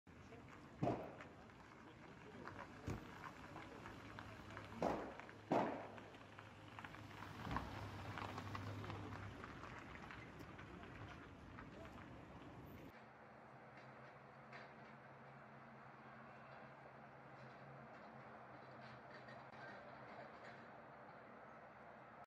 Крупный пожар на СТО в Петербурге удалось локализовать
Мощный пожар произошел в районе станции Старая деревня в Санкт-Петербурге. Возгорание зафиксировано на территории СТО.